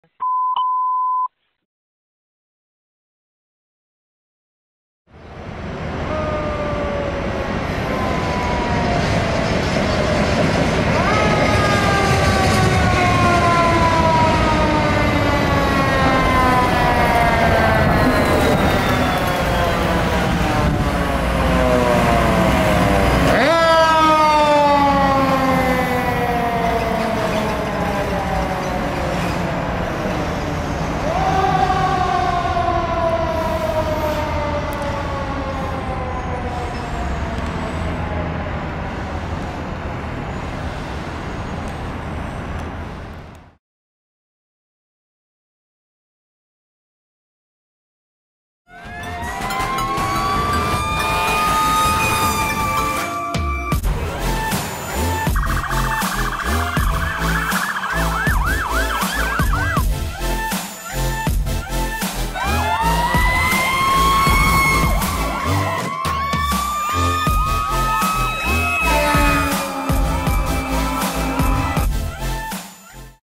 District of Columbia Fire & EMS Truck 3 Responding
District of Columbia Fire & EMS Truck 3 Responding to a fire alarm in Washington D.C.